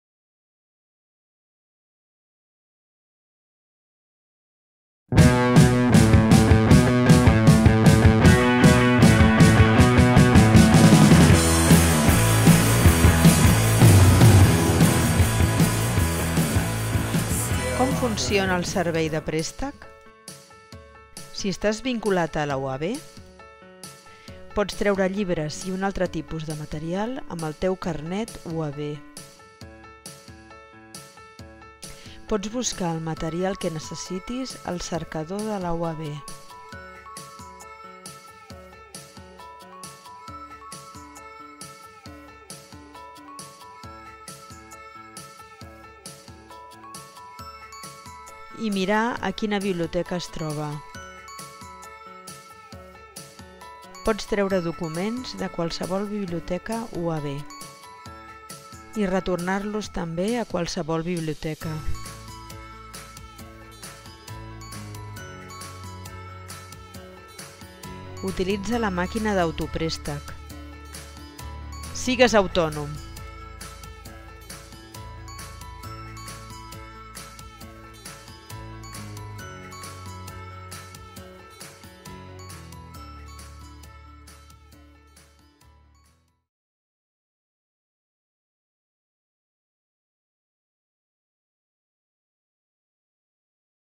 Com funciona el servei de préstec?
Vídeo on s'explica el servei de préstec de les biblioteques UAB.